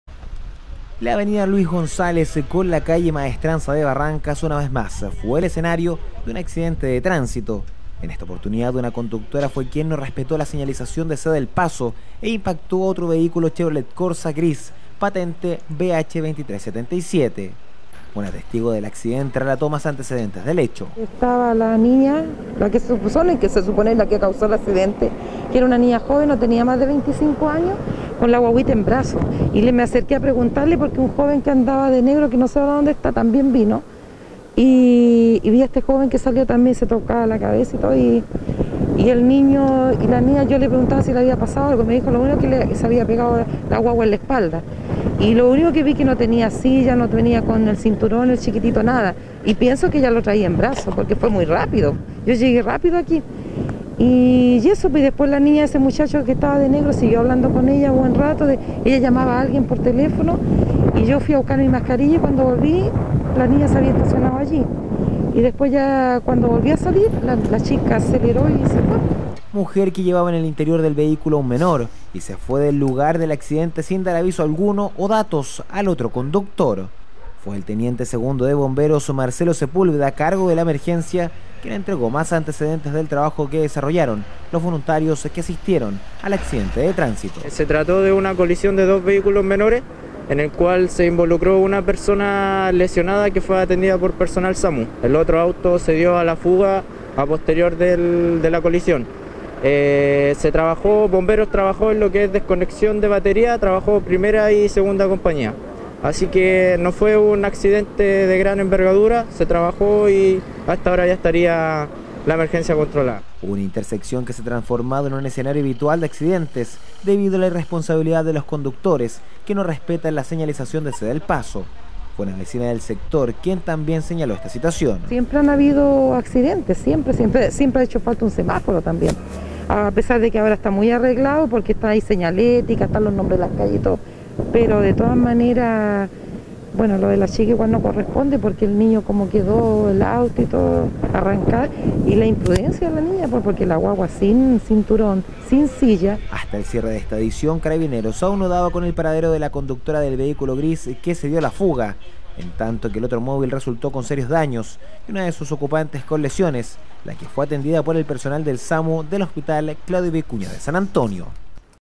06 OCT 2020 ACCIDENTE CAUSA EXPECTACIÓN EN BARRANCAS
LA TARDE DE ESTE MARTES SE PRODUJO EL CHOQUE DE DOS VEHÍCULOS EN LA INTERSECCIÓN DE LAS CALLES MAESTRANZA CON LUIS GONZÁLEZ  EN BARRANCAS DONDE UNA CONDUCTORA SE DIO A LA FUGA, TESTIGOS DEL HECHO SEÑALAN QUE LA MUJER QUE NO RESPETÓ EL CEDA EL PASO LLEVABA EN EL MÓVIL A UN PEQUEÑO. VECINOS DENUNCIAN QUE ESTA INTERSECCIÓN CONSTANTEMENTE SE TRANSFORMAN ACCIDENTES DE TRÁNSITO.